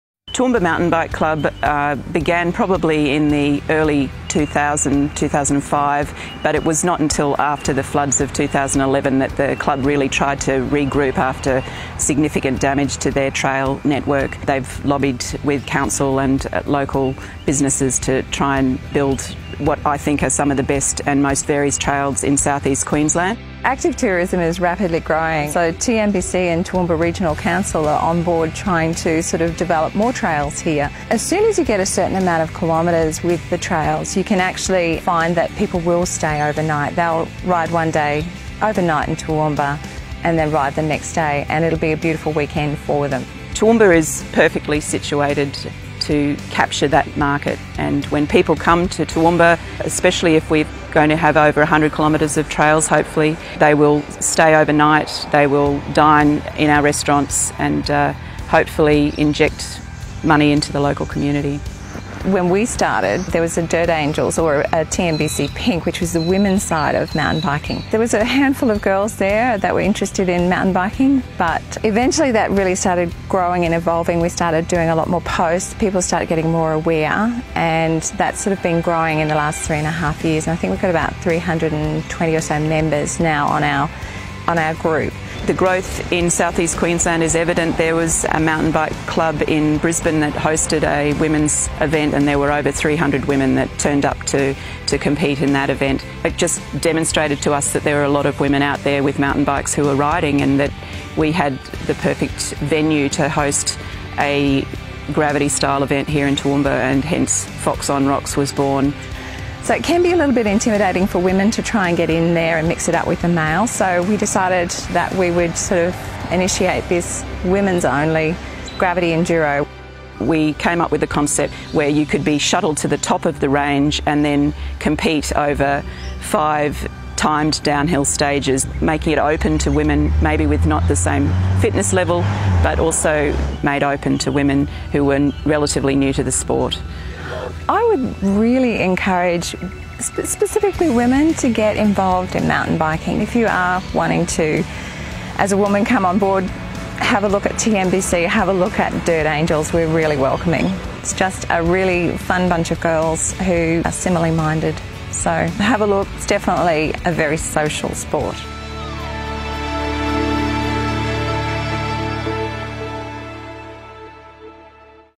Toowoomba Mountain Bike Club
Since 2004, we have been working with the Toowoomba Mountain Bike Club to create trails for mountain biking.
Over the coming months, we are set to host local, state and national competitions right here in our region.